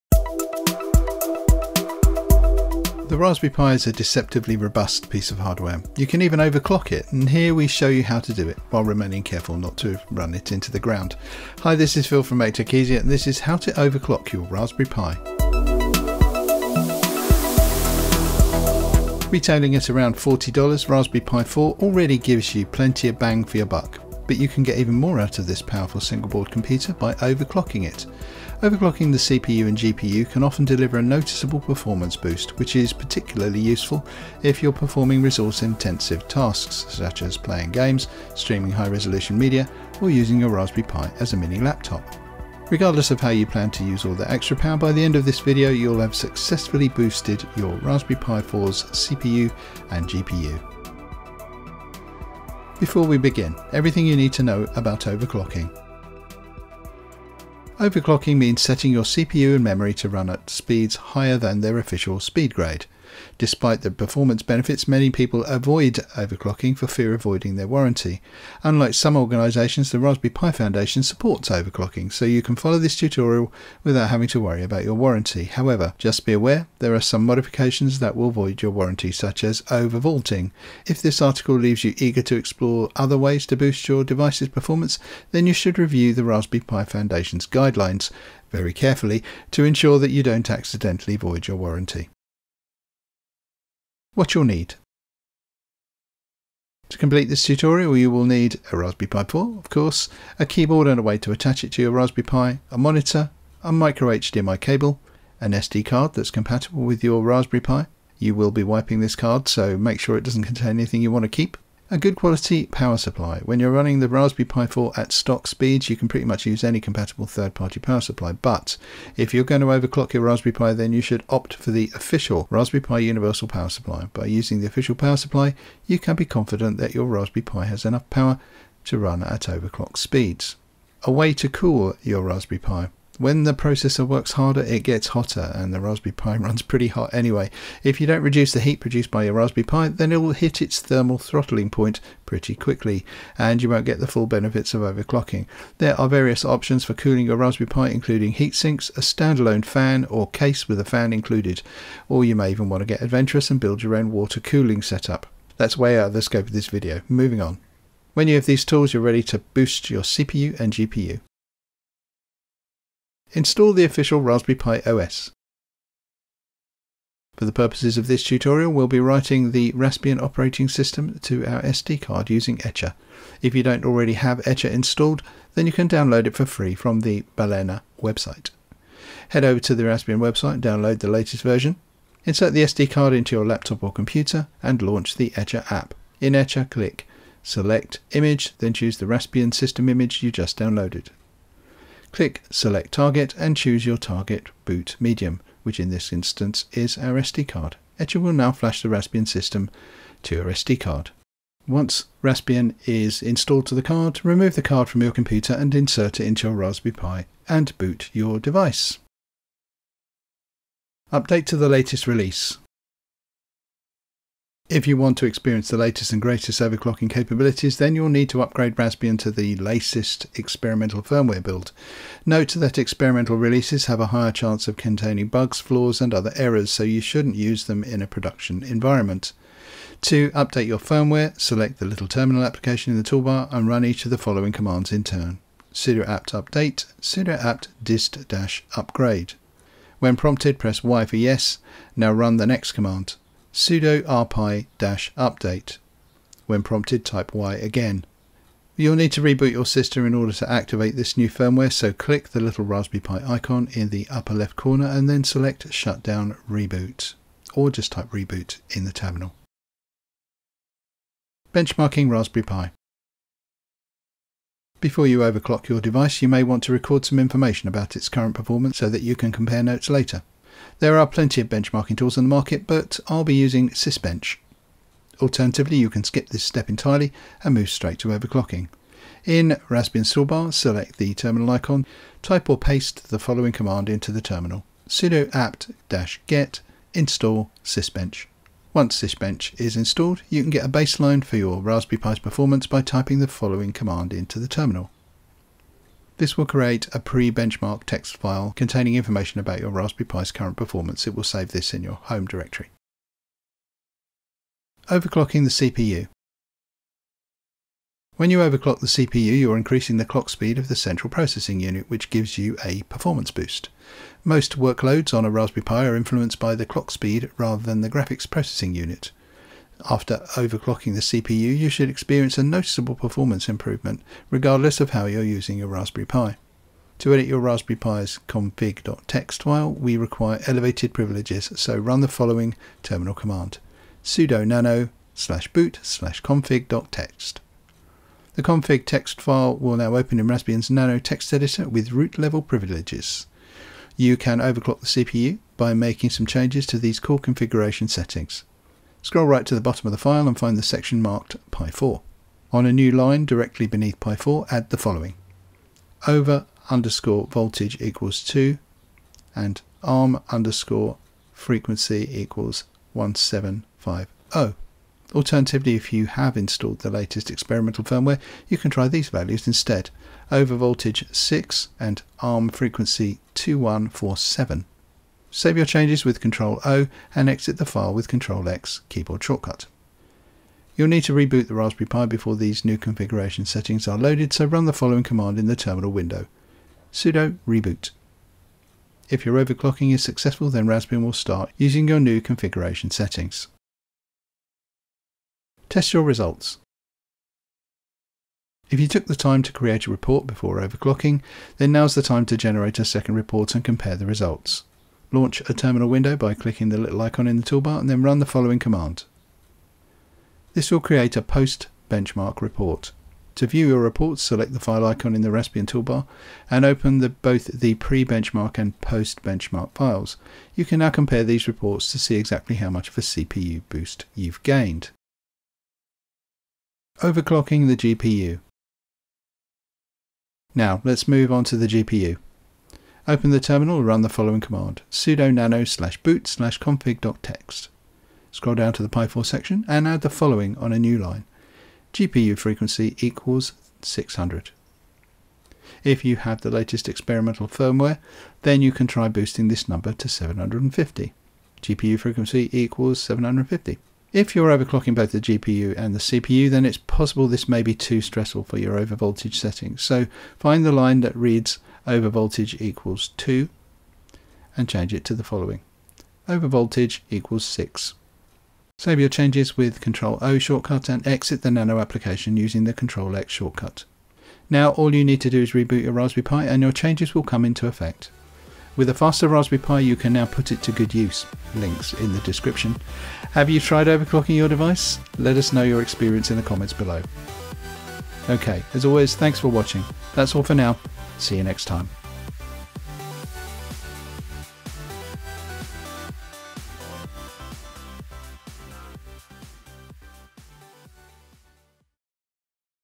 How to Overclock your Raspberry Pi
The Raspberry Pi is a deceptively robust piece of hardware. You can even overclock it, and here we show you how to do it (while remaining careful not to run it into the ground).